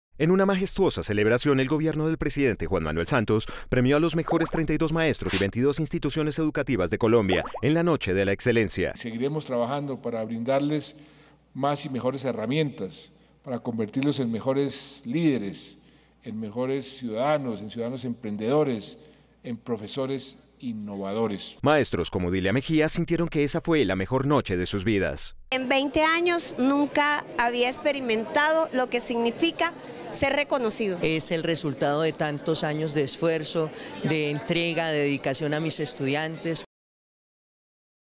La Noche de la Excelencia exaltó a los mejores maestros del país
El presidente de la República, Juan Manuel Santos, y la ministra de Educación, Gina Parody, reconocieron este 23 de noviembre a los mejores 32 maestros y 22 instituciones educativas de Colombia en La Noche de la Excelencia.